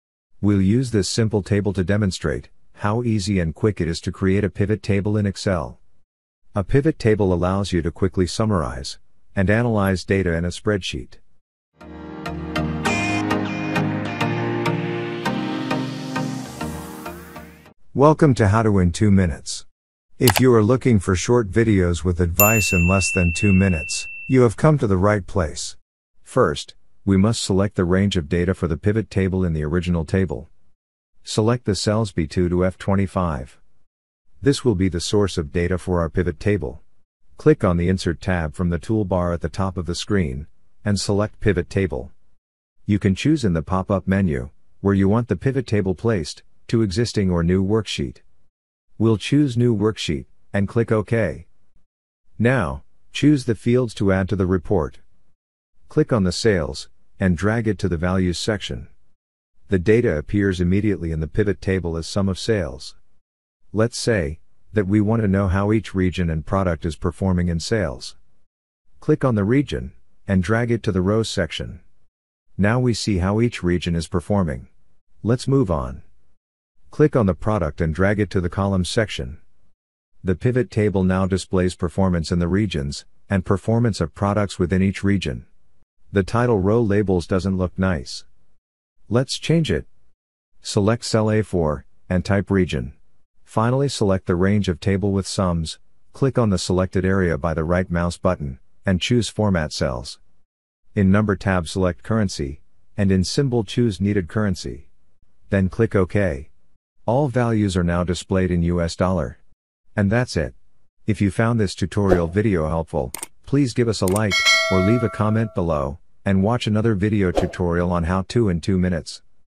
How to create a Pivot Table in Excel (Quick and Easy)
Learn quick and easy how to create a Pivot Table in Excel and save your valuable time. We will guide you through video and demonstrate it in very simple and easy way.

We'll use simple table to demonstrate how easy and quick it is to create a pivot table in Excel. A pivot table allows you to quickly summarize and analyze data in a spreadsheet.